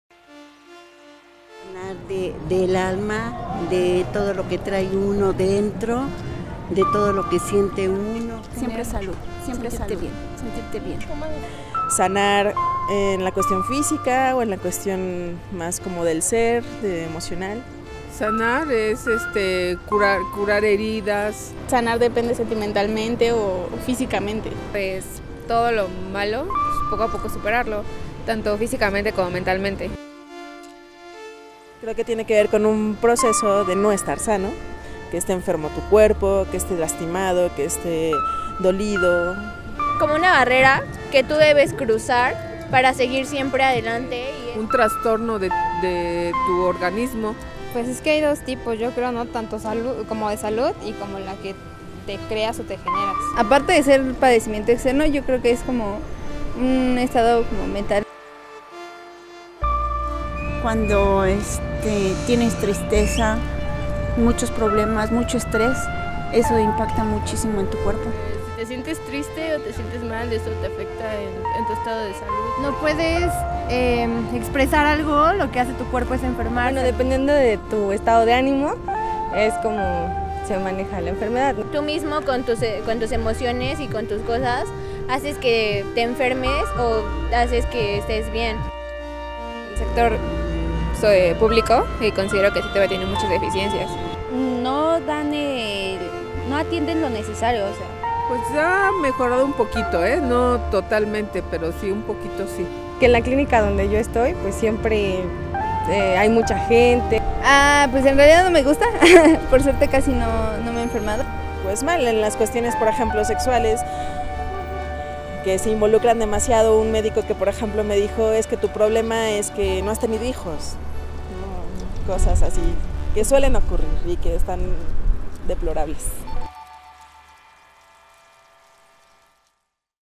SanarEs. . .
Para ti ¿qué es sanar?

Le preguntamos a varias mujeres de la ciudad de México sobre como viven la salud y la enfermedad.
Este vídeo forma parte de la serie de salud de Luchadoras MX